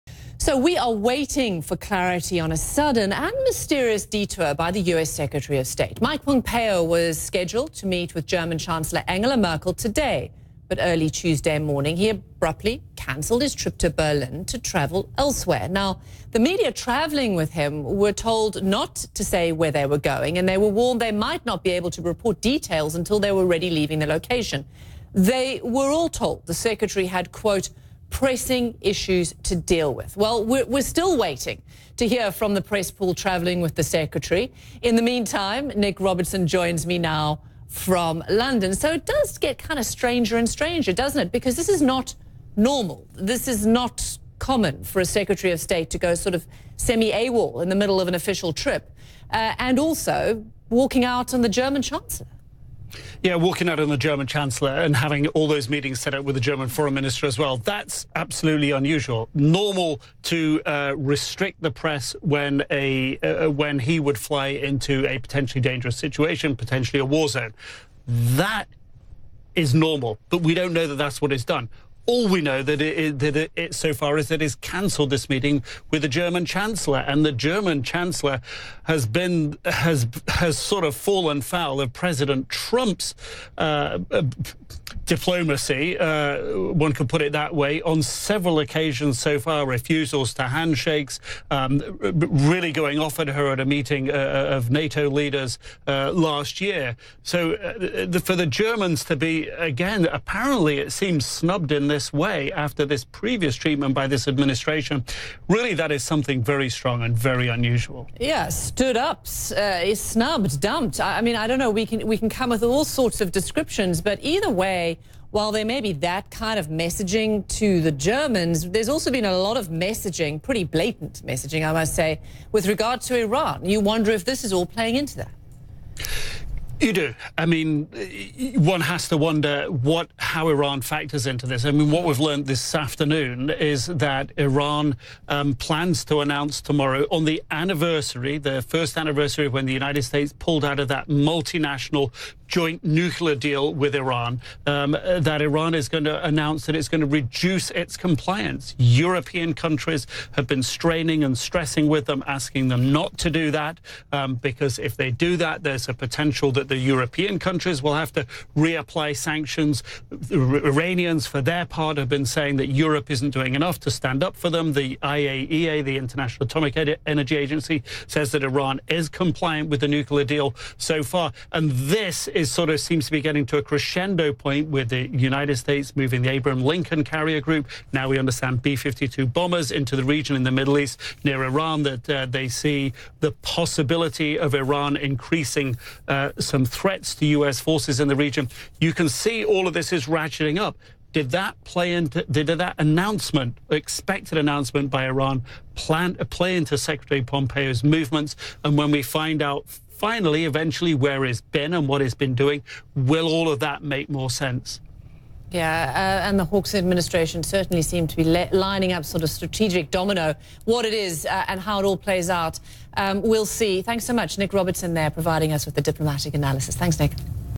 Pompeo sparks speculation by abruptly cancelling trip
Pompeo sparks speculation by abruptly canceling trip